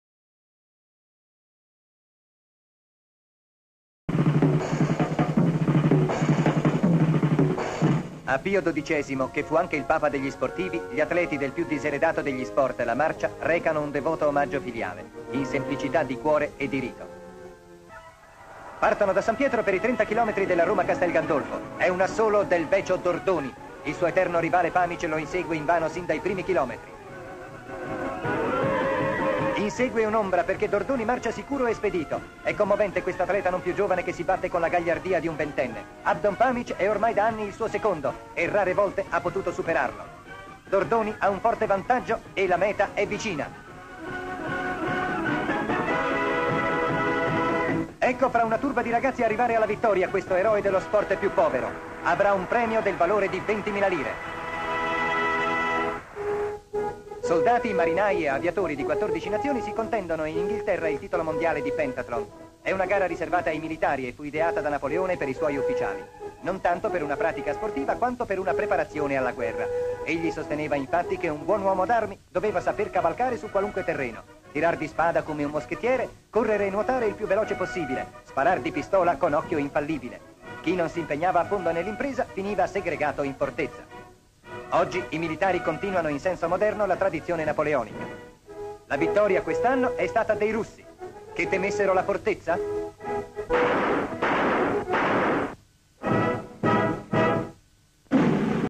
La vita sportiva
Corridori depongono corone di fiori alla tomba di Pio XII prima di mettersi in marcia
    La gara mondiale di pentathlon in Gran Bretagna.
   Descrizione sequenze:piazza san pietro, atleti portano corone di fiori alla tomba di Pio XII ; da piazza san pietro ha inizio la gara di marcia ; i marciatori passano accanto al colosseo, Pamich rincorre dordoni ; pamich corre sull'appia, si rinfresca la testa con la spugna offertagli da un allenatore in bicicletta ; dordoni in vantaggio seguito da ciclisti ; Dordoni entra a castelgandolfo, la popolazione nei vicoli del paese applaude il corridore ; dordoni rallenta ; Inghilterra ; concorrenti in prove di ippica, scherma, nuoto ; concorrenti nuotano nella piscina ; concorrenti sparano su obiettivi. ;